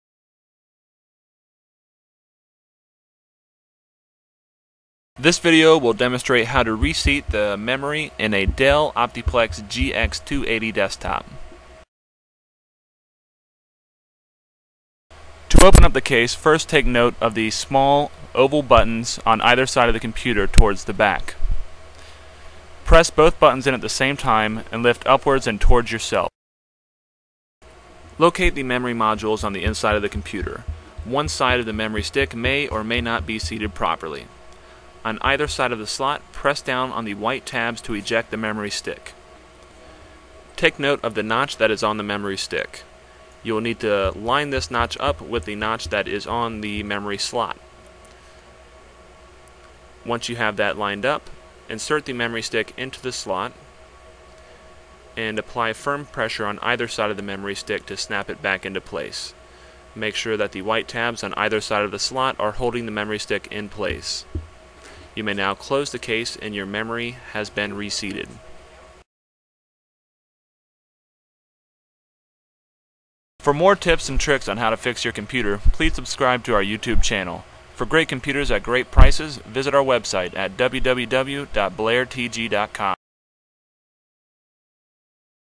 Tutorial: Reseat RAM on Dell OptiPlex GX280
A quick guide to reseating the memory modules on the Dell Latitude GX280.